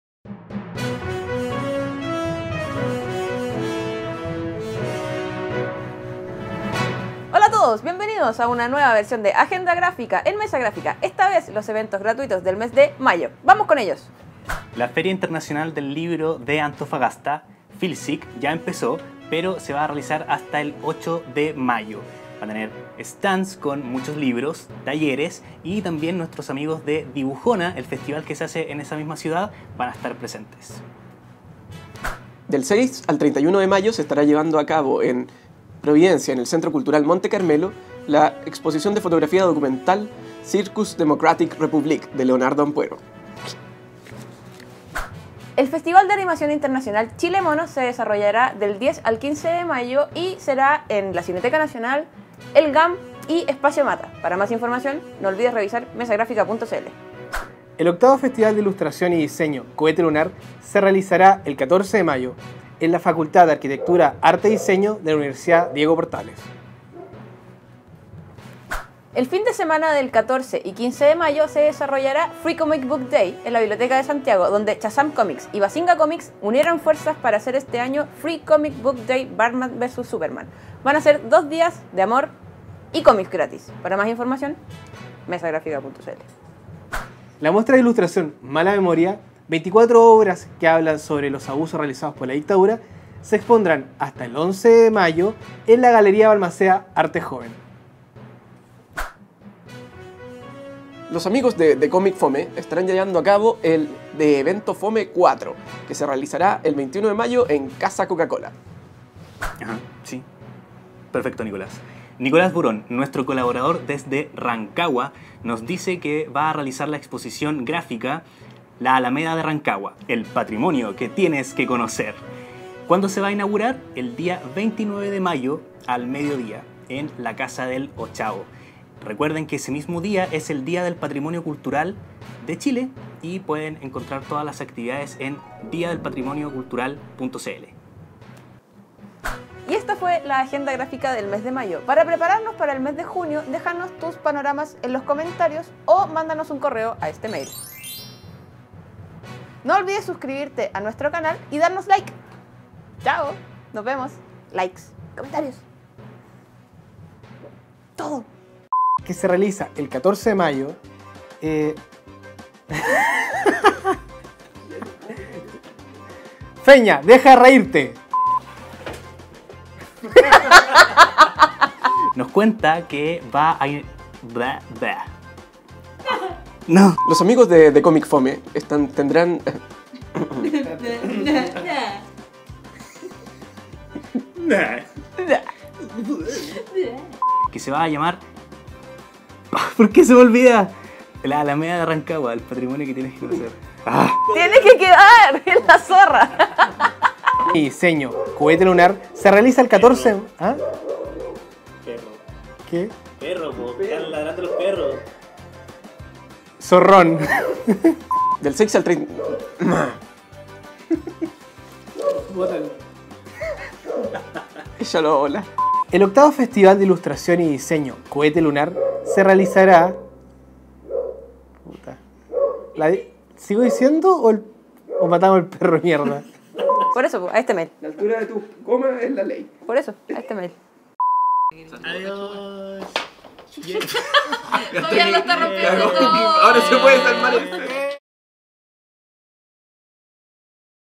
AgendaGráfica - Panoramas gratuitos Mayo 2016 | Mesa Gráfica
¿No sabes qué hacer durante mayo? ¡AgendaGráfica ya está aquí para ayudarte! Tenemos los mejores panoramas culturales gratuitos del mes. ¿Y cuál es la mejor parte? ¡Ustedes nos ayudaron a realizarla! Muchas gracias a todos por sus datos y recuerden que nos pueden dejar información para el mes de JUNIO en los comentarios. ¡Disfruten AgendaGráfica!

Agradecimientos y locación:
Taller Holanda